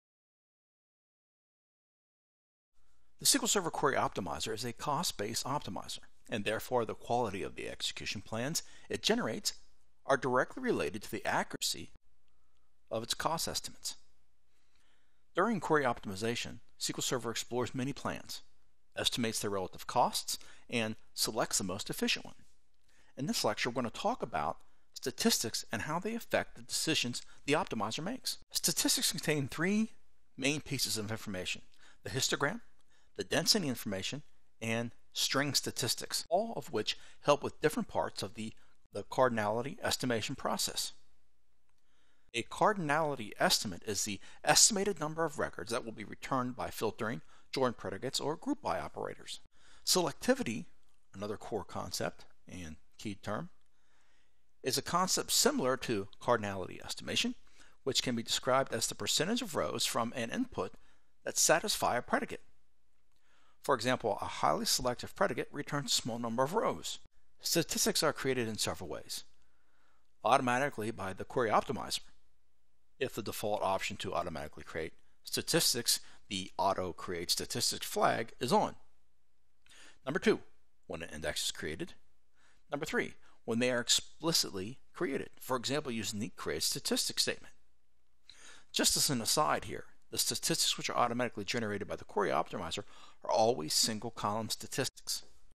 Mastering the Basics of SQL Server Query Optimization - DBCC SHOW STATISTICS
Statistics Contain 3 Key Pieces of Information in DBCC SHOW STATISTICS